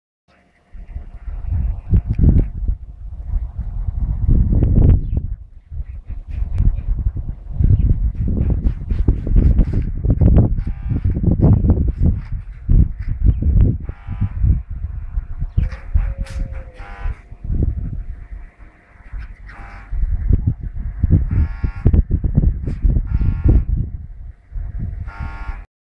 South Russian Ovcharka
First day at her new home :)